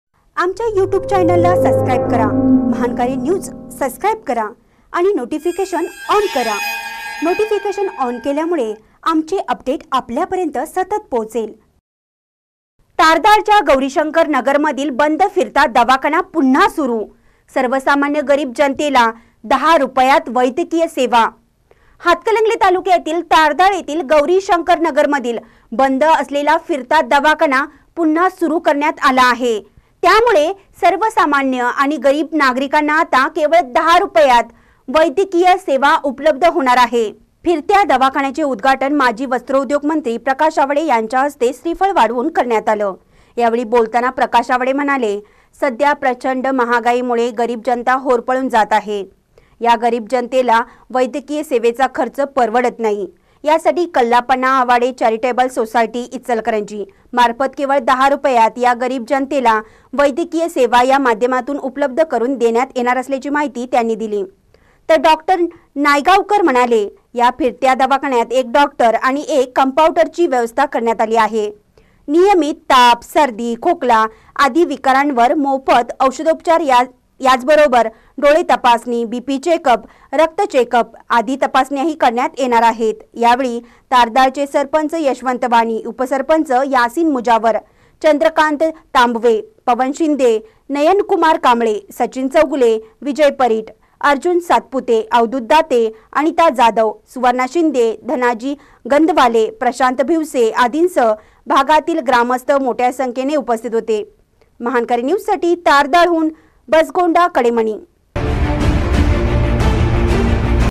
तारदाळच्या गौरीशंकर नगरमधील बंद फिरता दवाखाना पुन्हा सुरु
तारदाळच्या गौरीशंकर नगरमधील बंद फिरता दवाखाना पुन्हा सुरु : सर्वसामान्य,गरीब जनतेला १० रुपयात वैद्यकीय सेवा